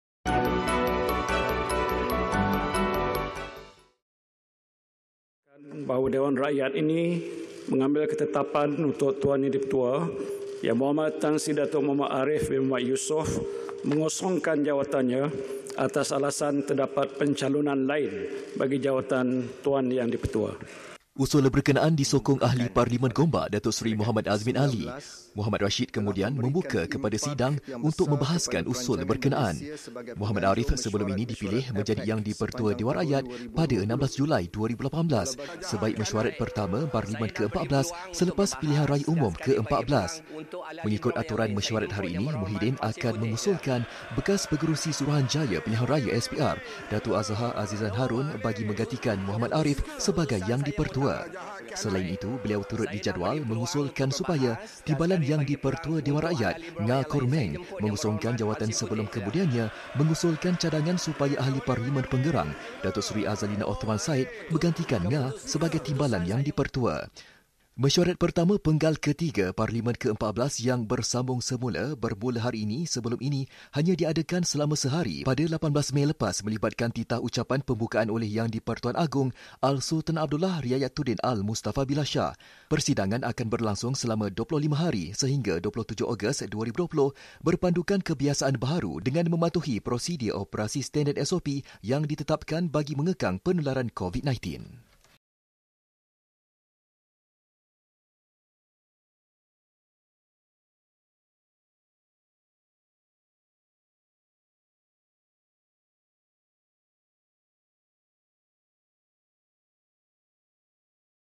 Parlimen : Muhyiddin kemuka usul pengosongan kerusi Yang Dipertua Dewan Rakyat
BuletinBernama : Perdana Menteri hari ini mengusulkan di Dewan Rakyat supaya jawatan Yang Dipertua Dewan Rakyat Tan Sri Mohamad Ariff Md Yusof dikosongkan atas alasan terdapat pencalonan lain bagi jawatan berkenaan.

Ahli Parlimen Pagoh itu mengusulkan cadangan tersebut di hadapan Timbalan Yang Dipertua Dewan Rakyat Datuk Mohd Rashid Hasnon.